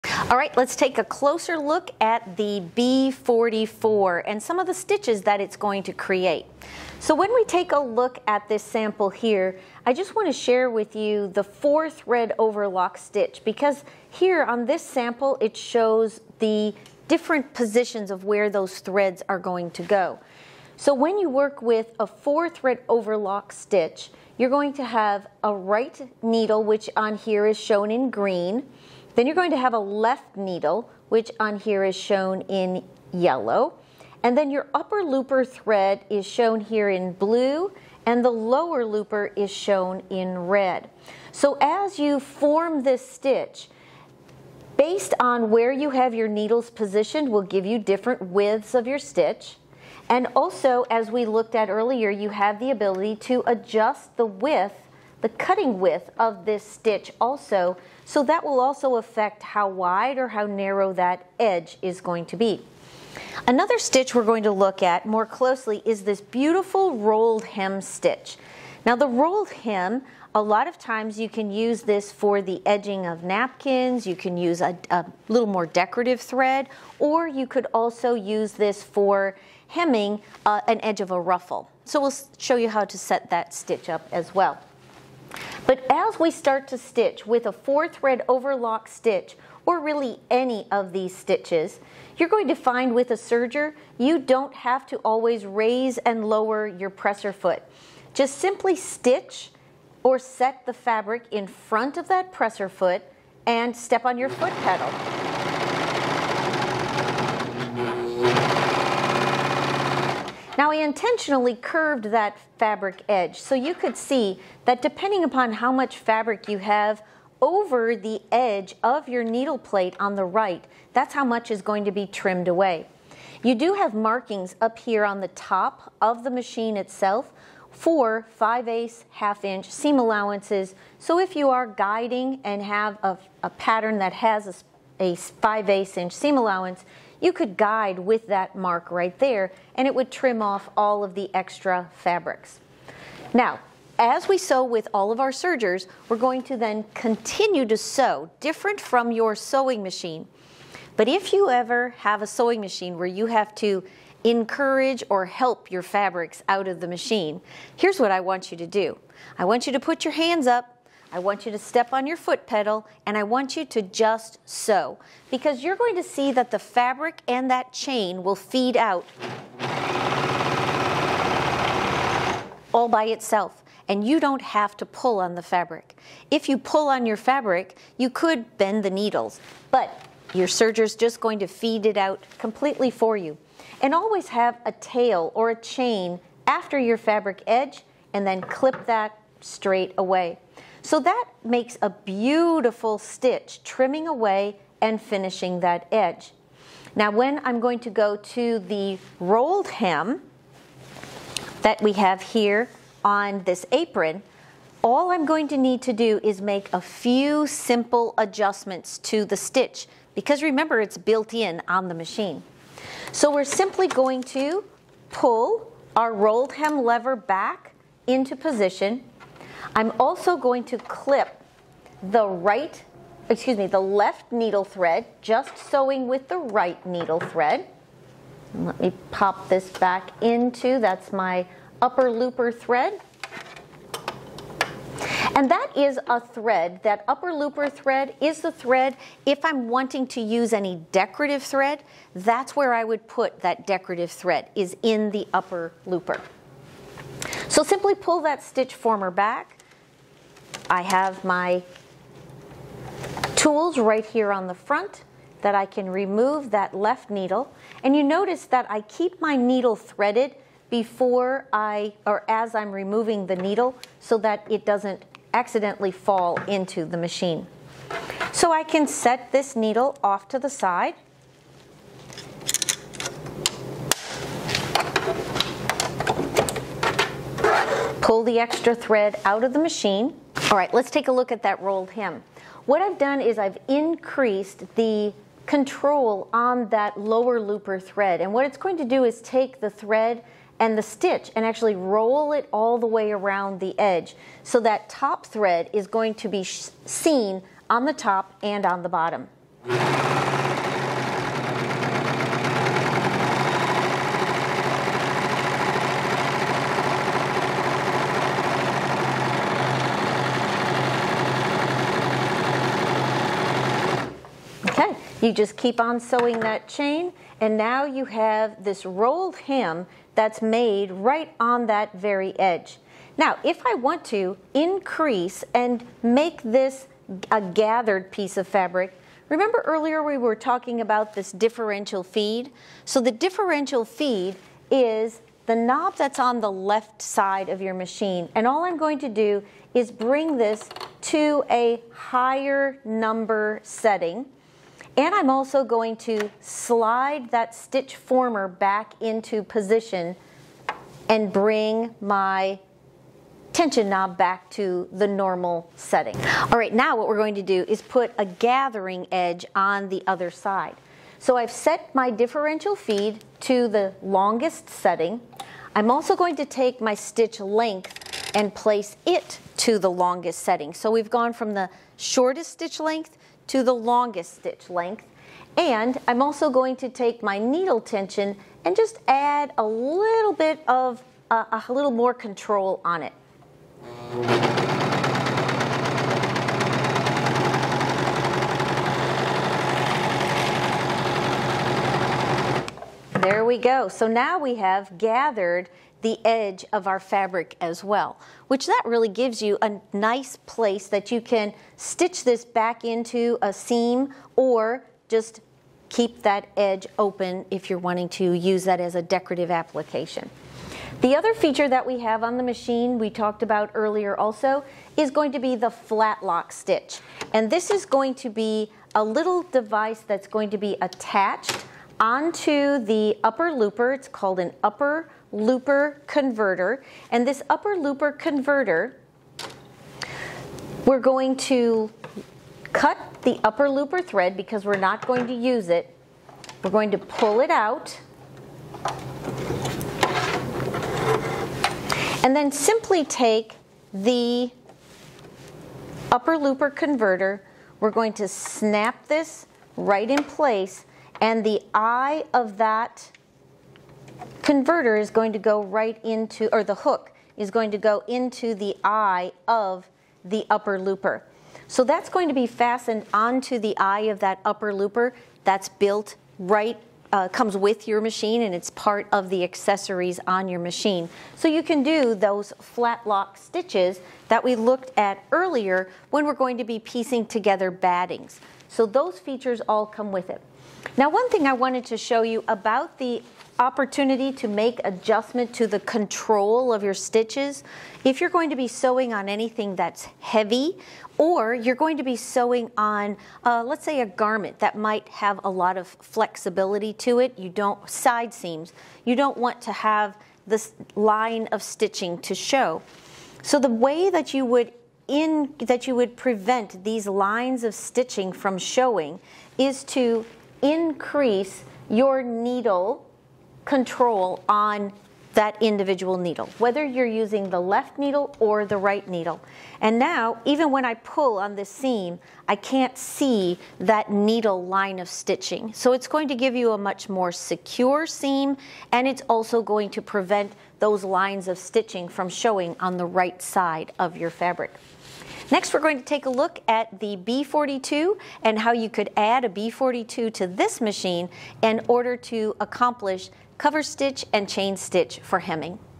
Bernette B44 Serger Feature Overview
In this video Pam Mahshie with Bernina and Ken's Sewing Center in Muscle Shoals, AL gives us a more in depth feature overview of the Bernette B44 serger.  You will be amazed at all you can do with this serger.

Ken's Sewing Center is an authorized Bernette dealer.  We carry all parts and accessories and can help you use them all.